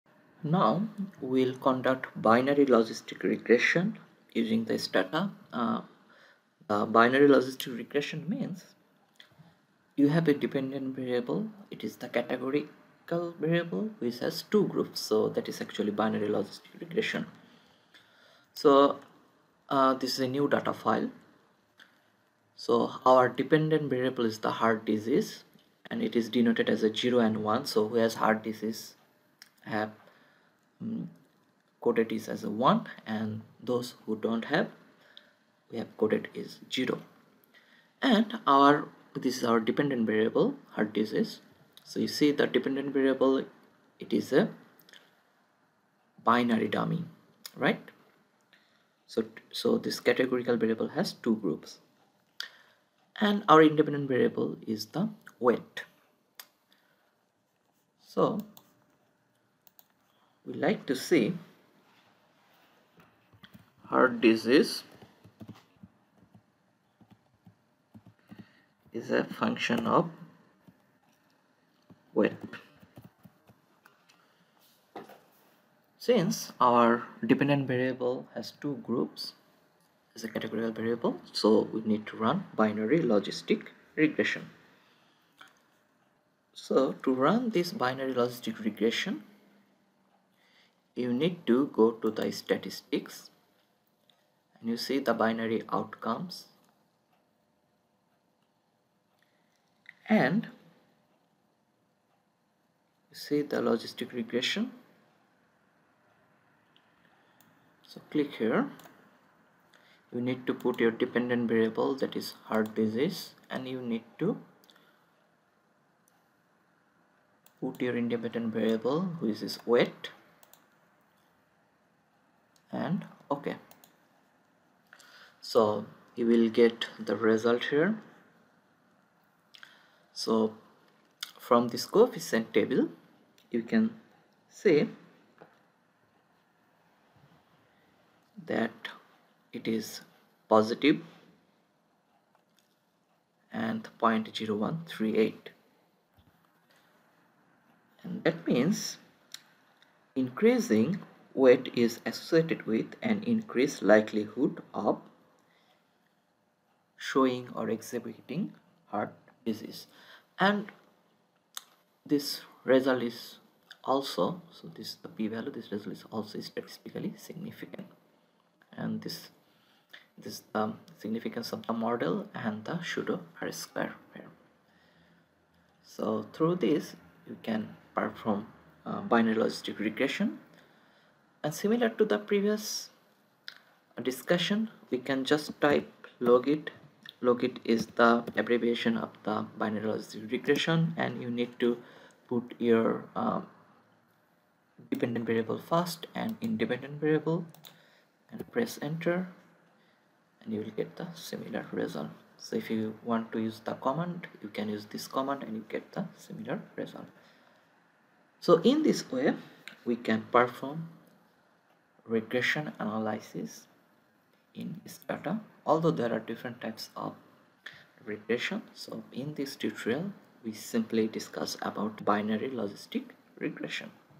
Performing simple logistic regression using Stata
Dewan, one of the Stats@Liverpool tutors at The University of Liverpool, demonstrates how to perform a simple logistic regression using Stata.